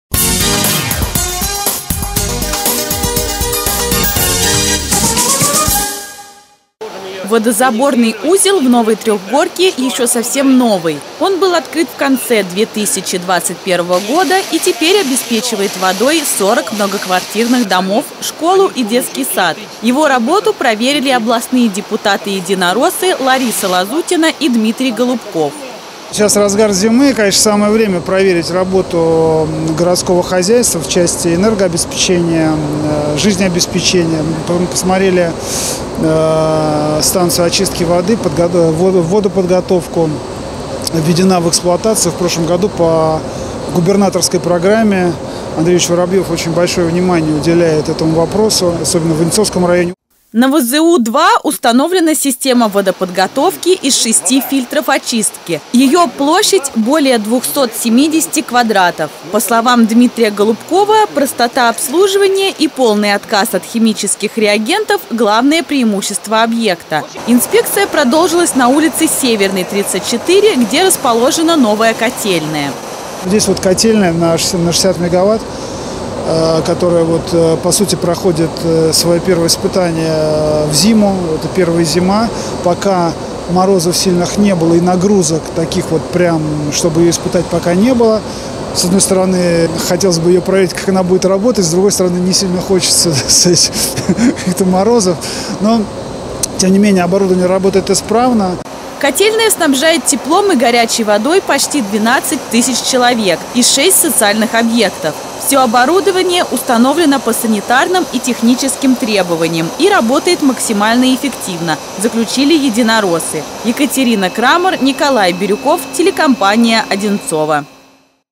Депутатская инспекция
Первый зампред Московской областной Думы Лариса Лазутина и ее коллега по партийной фракции Дмитрий Голубков проинспектировали котельную на улице Северная и станцию ВЗУ-2 в Новой Трехгорке.